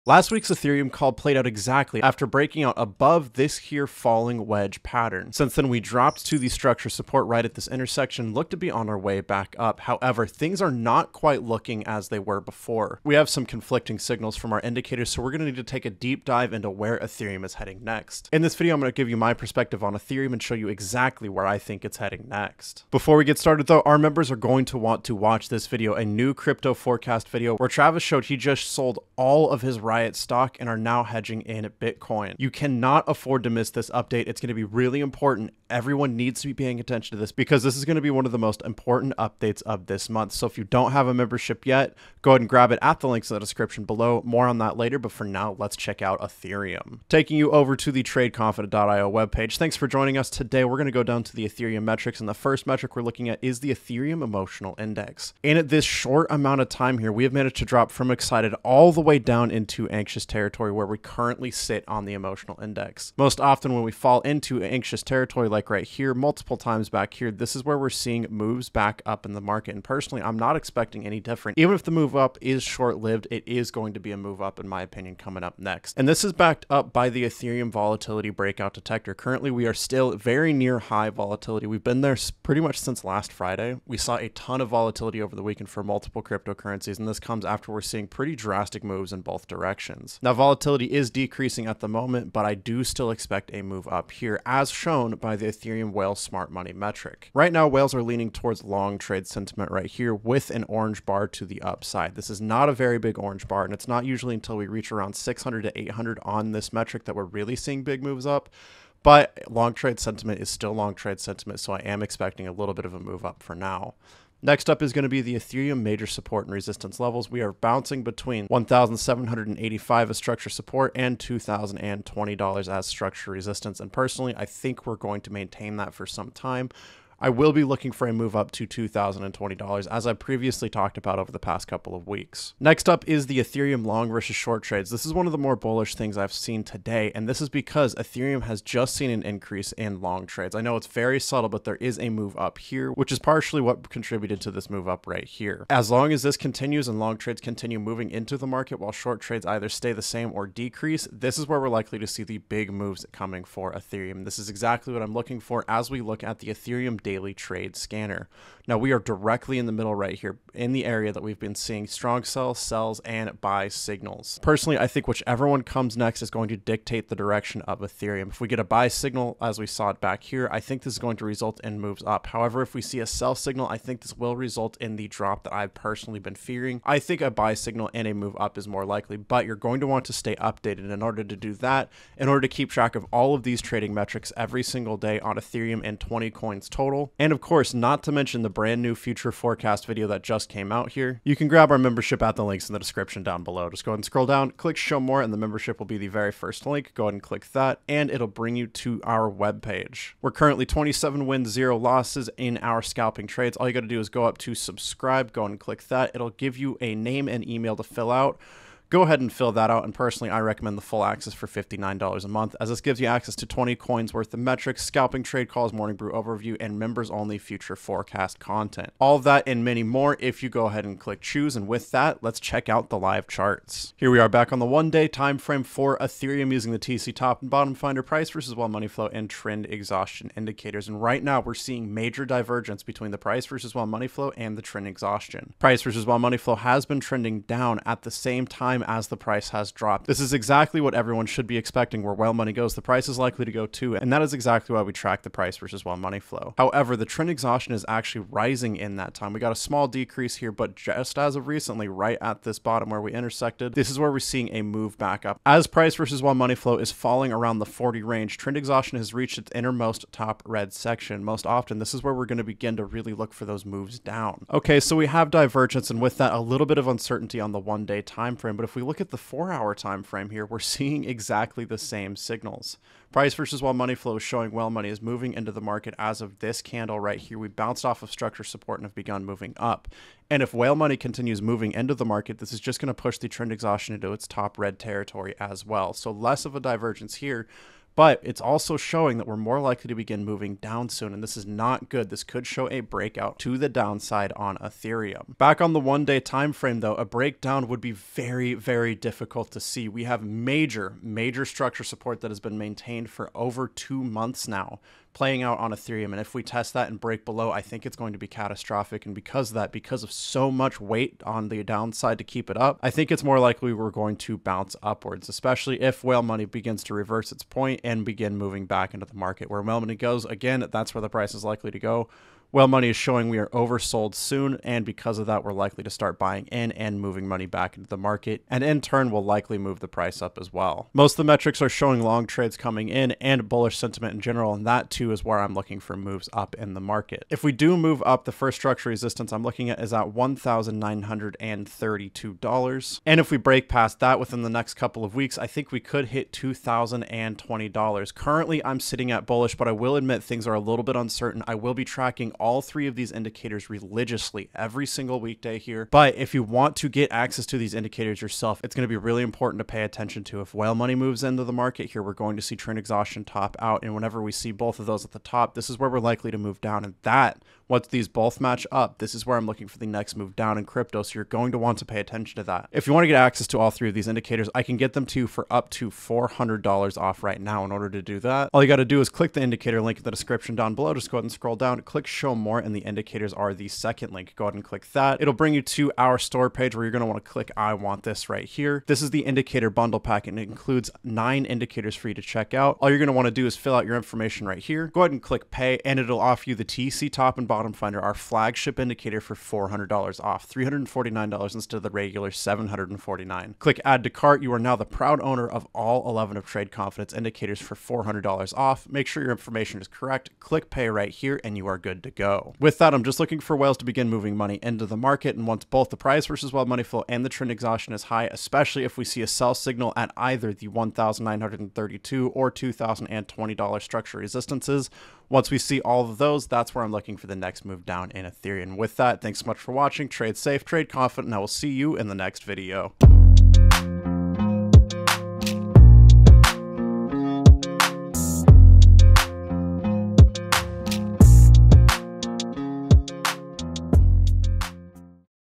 Ethereum DIVERGENCE!! (PUMP OR DUMP NEXT WEEK?) | ETH Price Prediction
In today’s Ethereum price prediction video, I'll show you how Ethereum is experiencing uncertainty as our indicators show conflicting calls. Whales are moving money into the market, but we are seeing a likely end to the recent uptrend. Will ETH see a 10% PUMP or a CRASH?

Time Stamps:
0:46 - Ethereum Metrics
3:43 - Ethereum Price Prediction

Video Topics